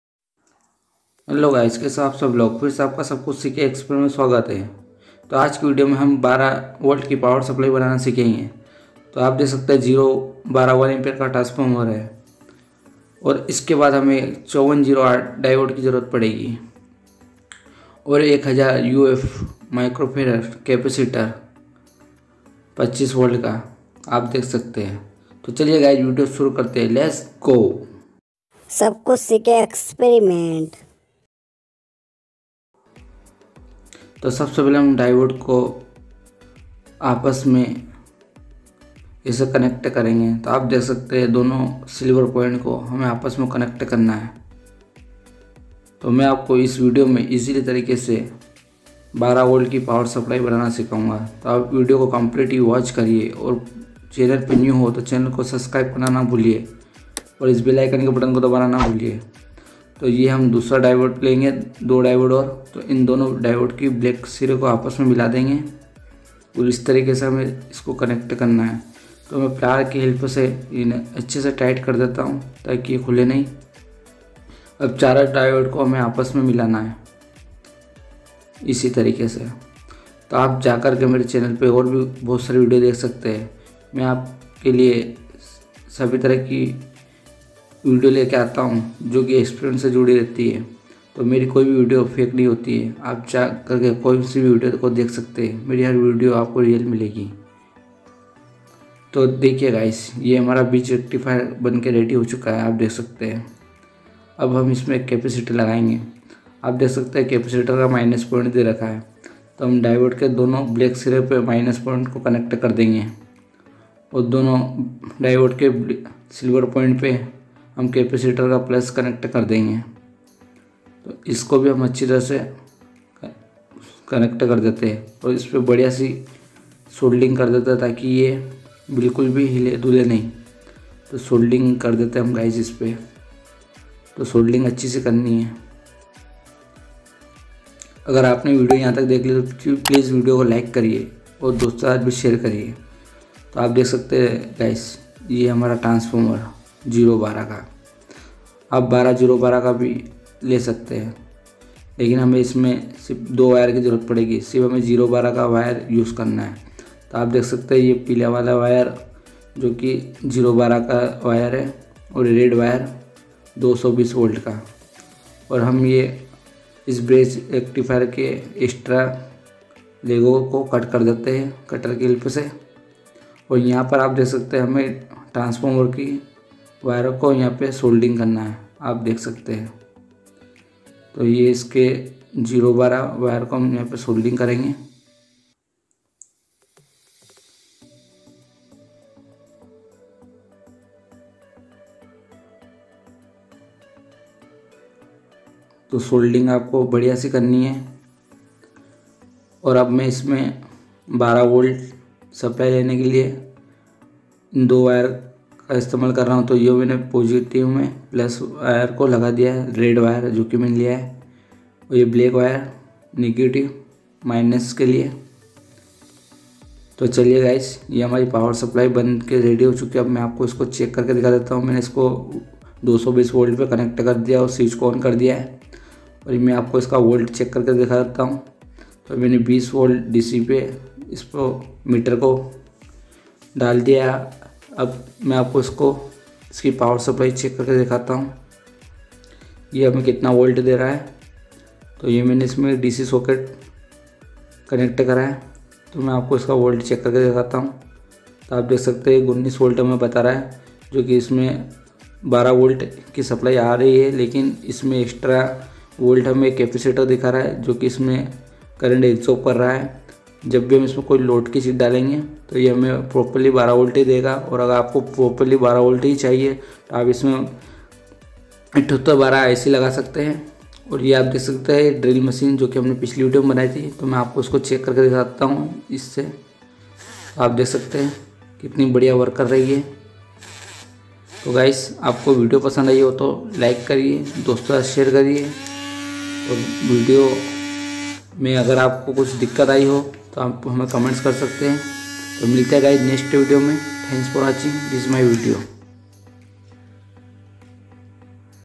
12 volt Dc supply (1A Transformer ) easyway
Hello guys 🤗
kese ho aap sab log fer se aap ka sab khuch sikhe experiment me sawagat hai..
aaj ke video me ham bat kare ge 12v dc power supply ba ne va le hai..

video aap ko pasand aye to please yaar..🙏

Electronic component:-
0 -12 1amp. transformer
1n 5408 Diode
25v 1000uf microferet capacitor

{ Good project video my channel }

milte hai next video 👋
{ Thinks for watching my video }